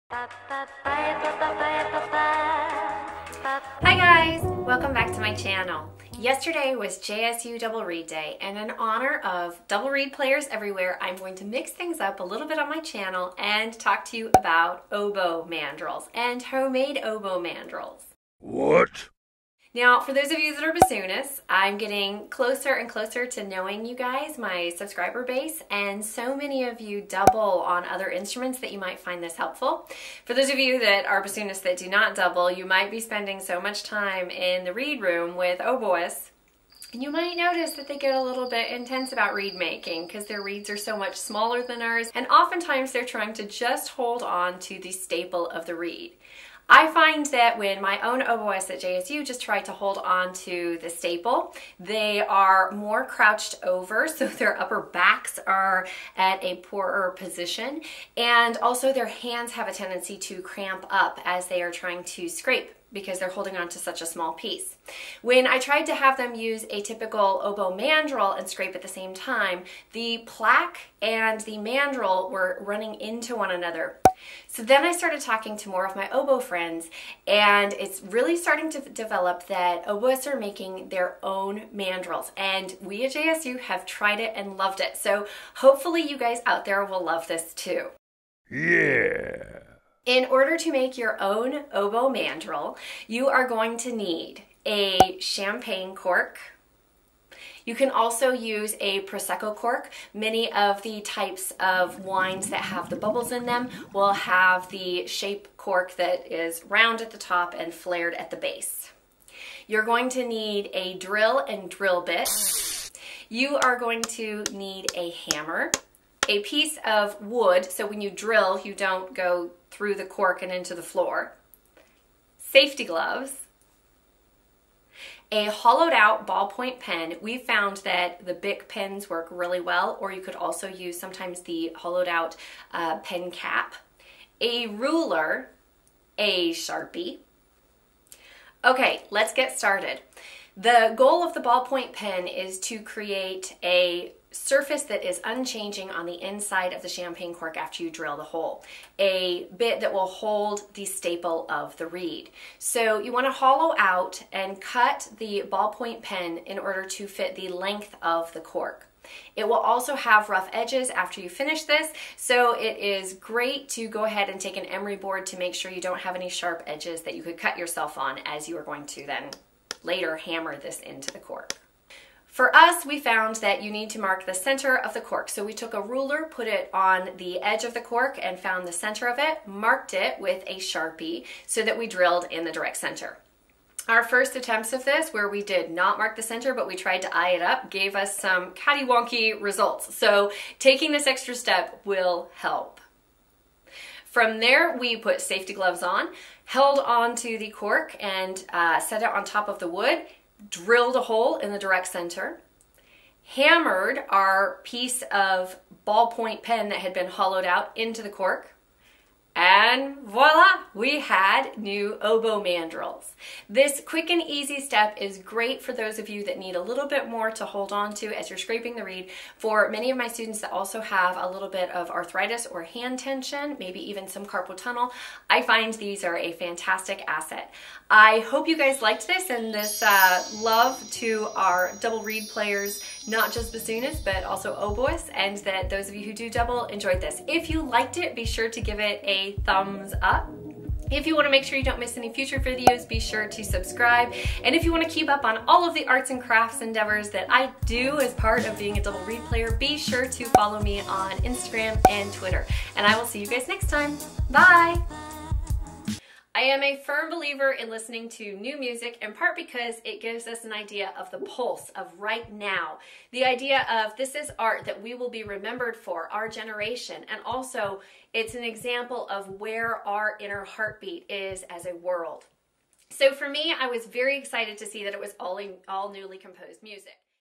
HOW TO: Making OBOE MANDRELS for SCRAPING REEDS
Hi Guys! In Honor of Yesterday's JSU DOUBLE REED DAY, I am sharing how we made oboe scraping mandrels. So many of you guys Double on DOUBLE REEDS, it seems fitting! Be sure to tag us in pic of your new scraping mandrel

♦ F I N D M E E R R ' W H E R E ♦